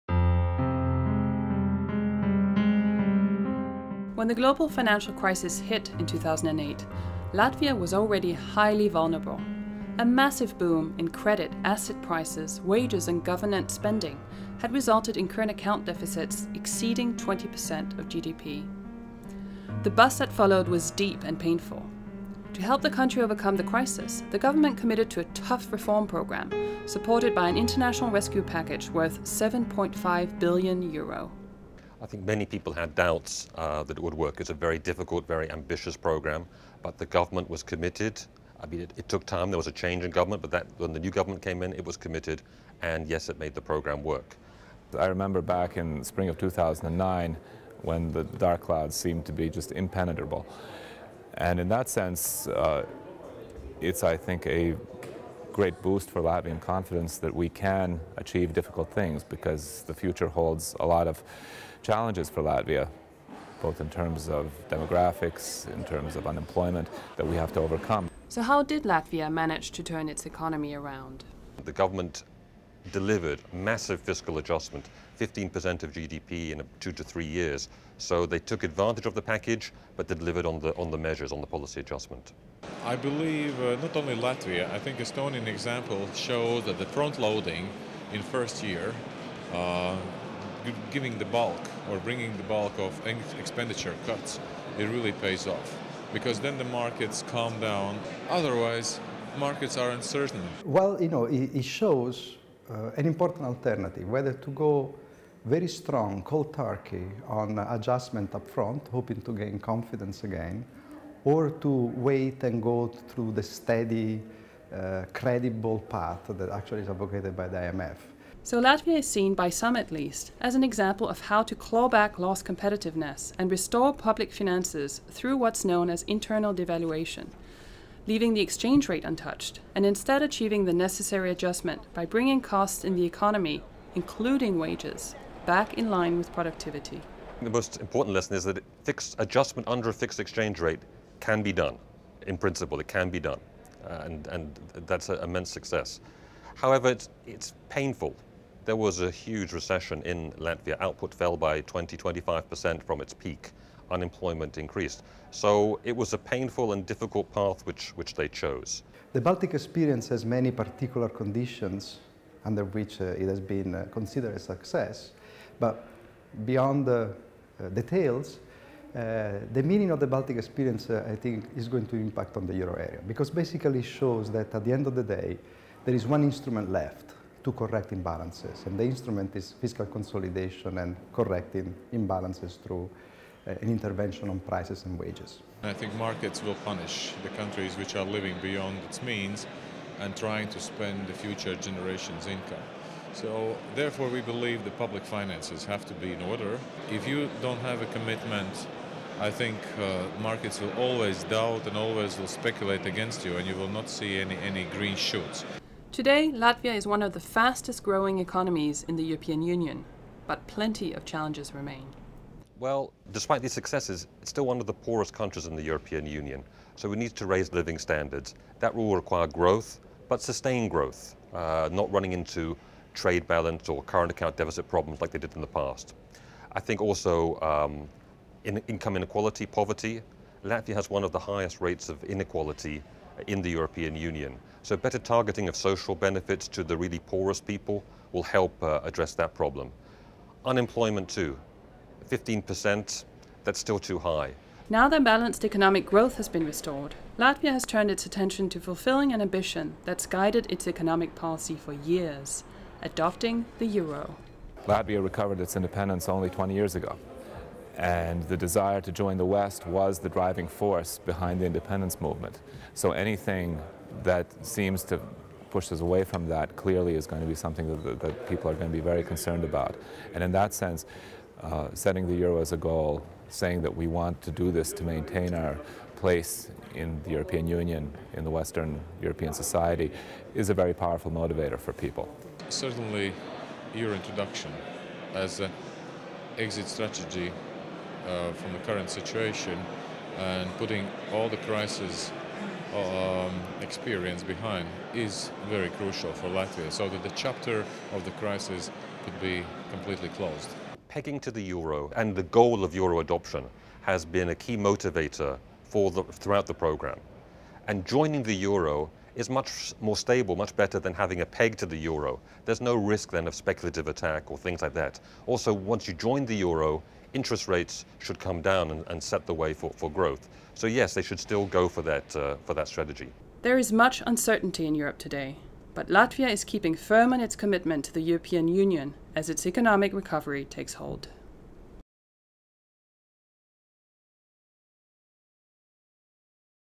How Latvia Recovered From the Crisis
Latvia was hit particularly hard by the global economic crisis. Today, however, it is one of the fastest growing economies in the European Union. How did Latvia overcome the crisis? And does its experience hold any lessons for other economies?

Speakers:

Mark Griffiths
Former IMF mission chief for Latvia

Pauls Raudseps
Commentator, weekly magazine "Ir"

Ilmars Rimsevics
Governor of the Bank of Latvia

Giancarlo Corsetti
Professor of Macroeconomics at Cambridge University